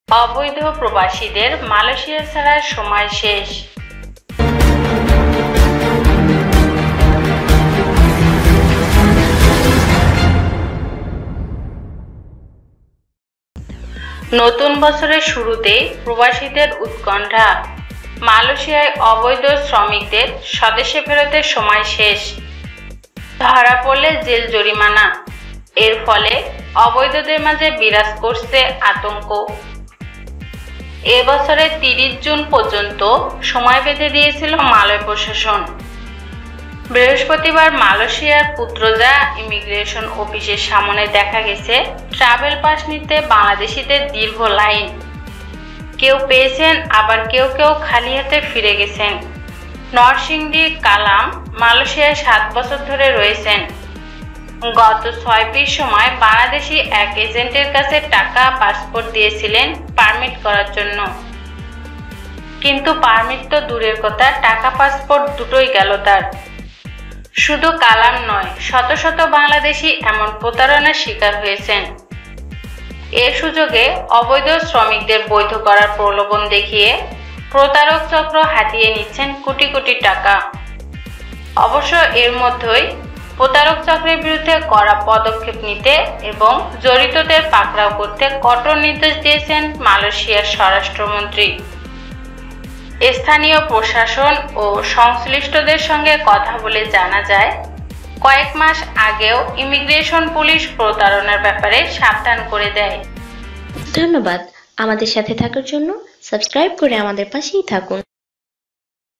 অবৈধ প্রবাসীদের মালয়েশিয়া ছাড়ার সময় শেষ । সকল মালেয়শিয়া প্রবাসী ভাই জানুন বিস্তারিত ।।
নতুন বছরের শুরুতেই প্রবাসীদের উৎকণ্ঠা। মালয়েশিয়ায় অবৈধ শ্রমিকদের স্বদেশে ফেরতের সময় শেষ। ধরা পড়লে জেল জরিমানা। এর ফলে অবৈধদের মাঝে বিরাজ করছে আতঙ্ক। এ বছরের ৩০ জুন পর্যন্ত সময় বেধে দিয়েছিল মালয় প্রশাসন। বৃহস্পতিবার মালয়েশিয়ার পুত্রাযায়া ইমিগ্রেশন অফিসের সামনে দেখা গেছে- ট্রাভেল পাস নিতে বাংলাদেশিদের দীর্ঘ লাইন।